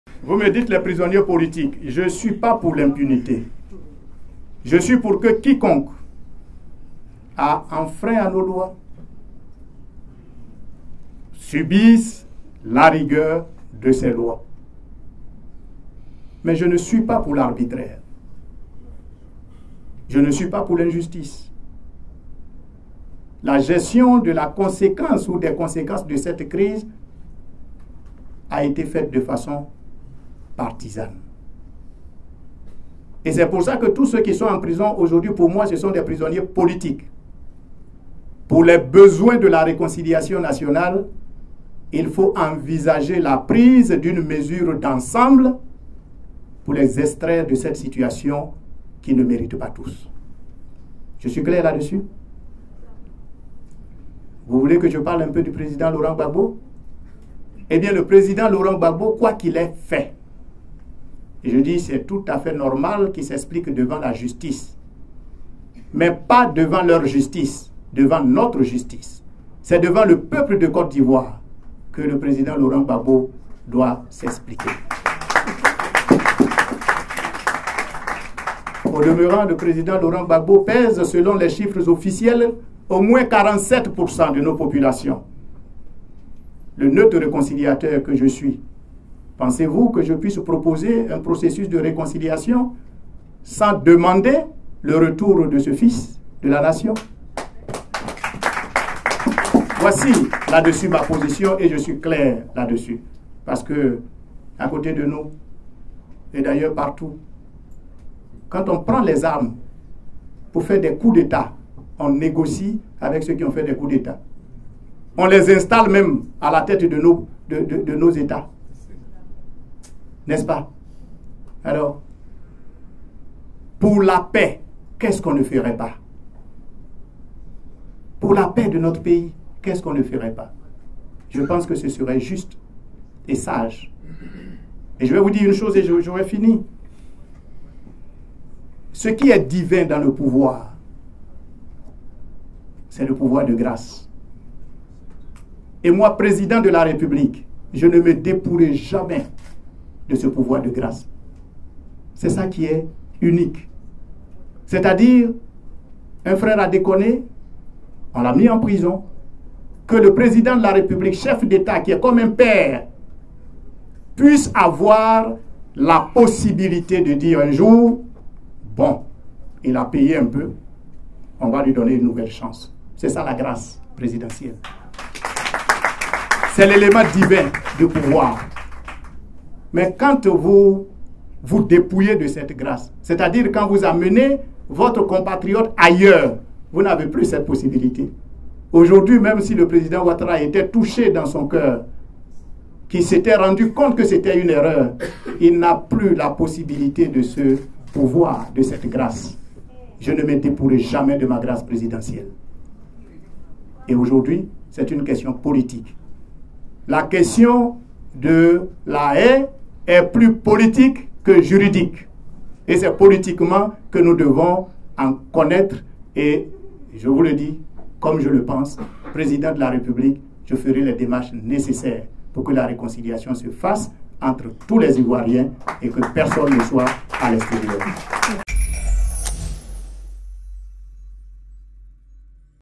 Siméon Kouadio: "Gbagbo devrait s’expliquer devant le peuple de Côte d’Ivoire"
Siméon Kouadio Konan, candidat indépendant à la présidentielle 2015, a affirmé mardi 22 septembre face à la presse, que l’ex-président de la République Laurent "Gbagbo devrait s’expliquer devant le peuple de Côte d’Ivoire" et "non devant leur justice".